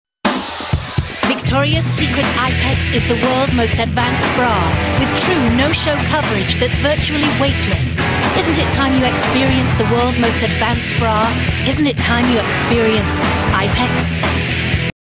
Alessandra Ambrosio commercial for IPEX bra
Supermodel Alessandra Ambrosio and other models in a Victoria's Secret TV commercial for the IPEX bra. 15-second version. First aired in the USA in March 2007